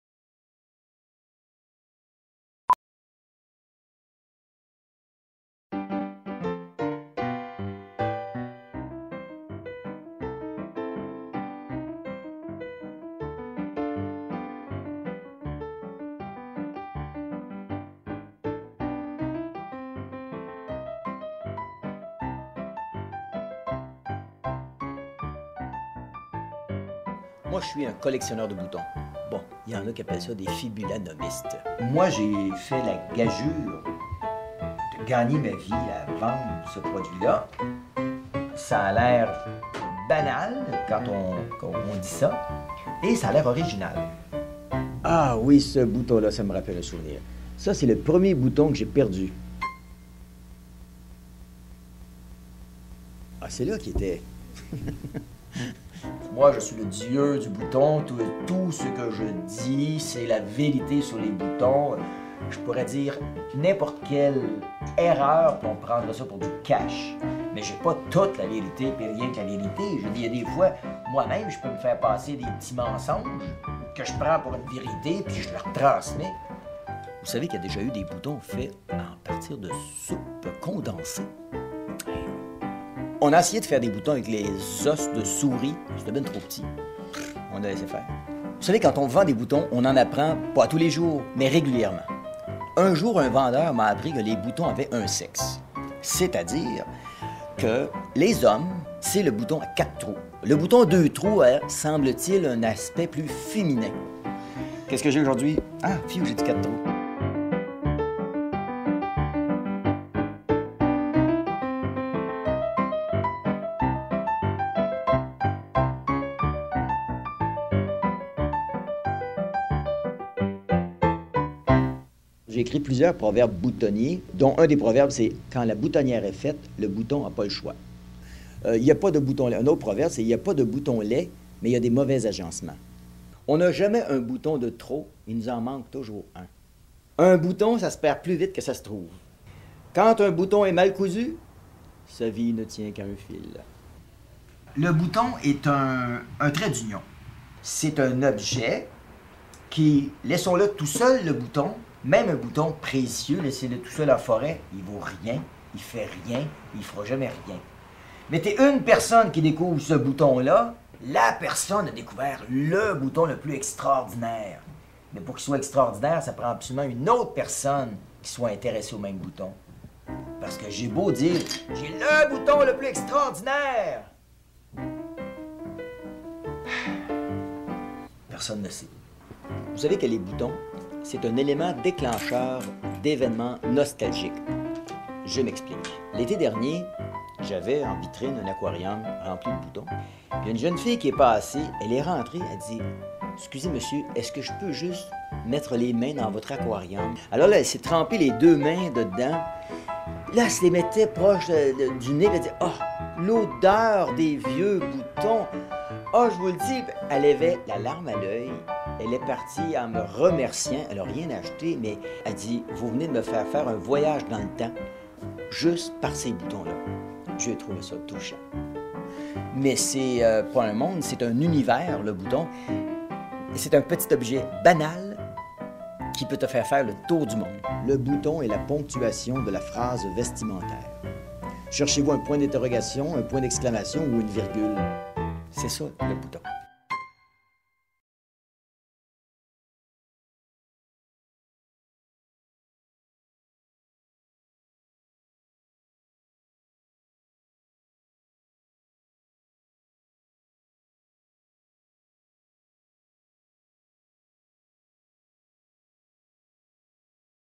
Fibulanomiste
Richard Letendre est un fibulanomiste, c'est-à-dire un collectionneur de boutons.

Richard Letendre is a fibulanomist, meaning that he collects buttons.